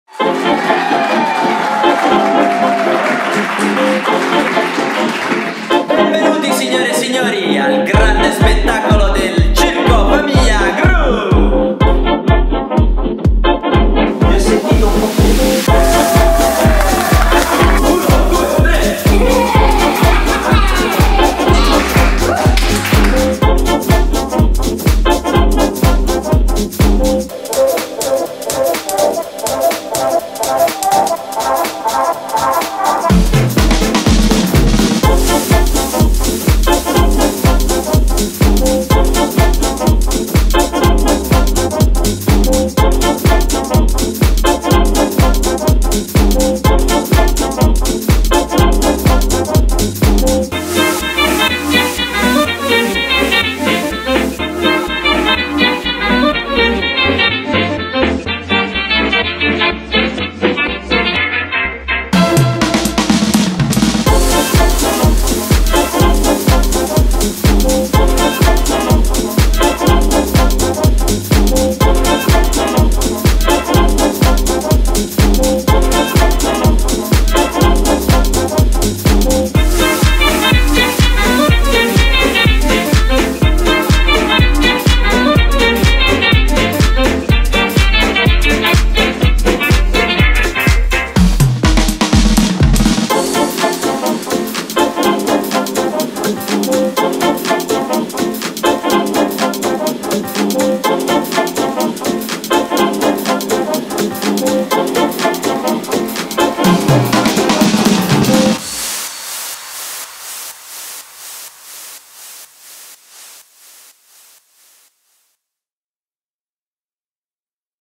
Circo Famigliagroup video promo
Un intero spettacolo di cabaret con un presentatore e varie gag comiche per grandi e piccini. Il pubblico potrà scoprire tutte le diverse abilità circensi di questa poliedrica e simpatica famiglia.